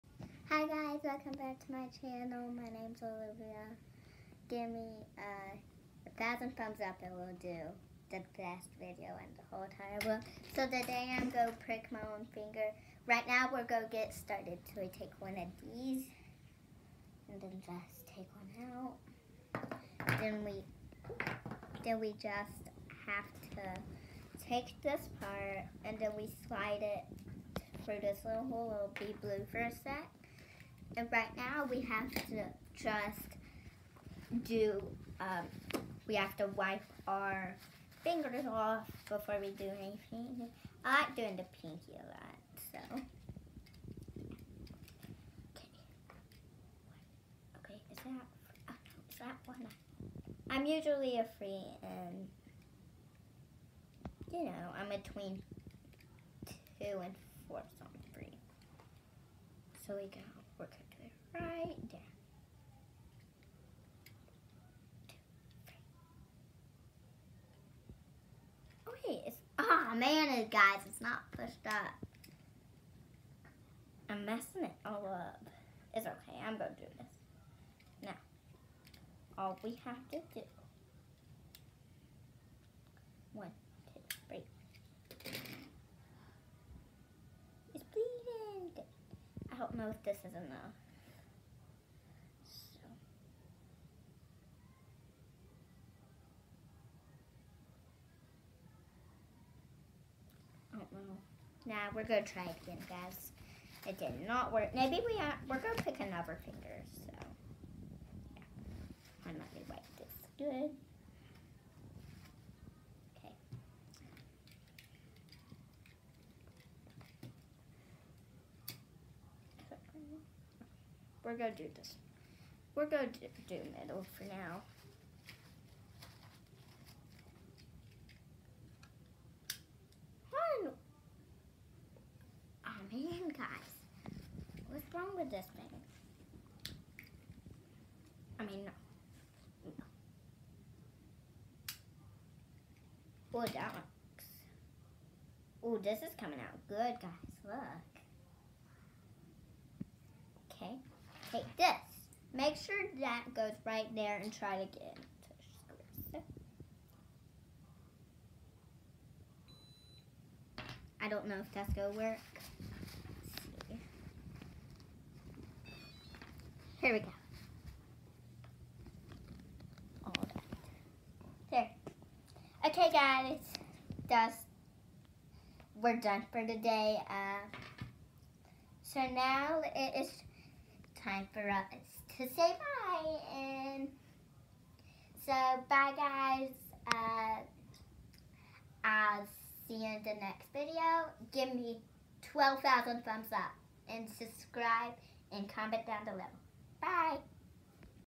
How to check your blood sugar
Olivia is 5 years old and has type 1 diabetes. This is a how to video on checking your blood sugar level